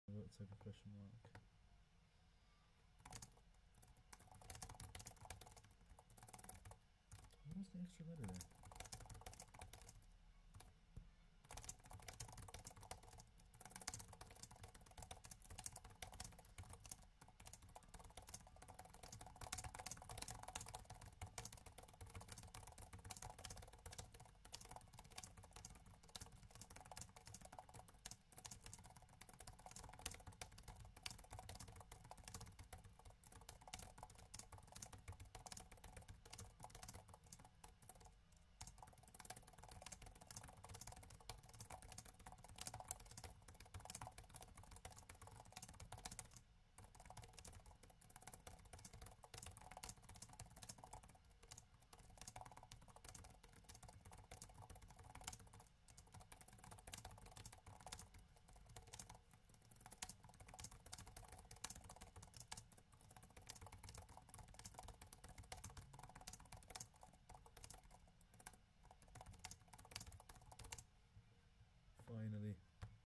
115 words per minute
I was able to get 115 words per minute on the typing test after an hour of trying to get this many words per minute.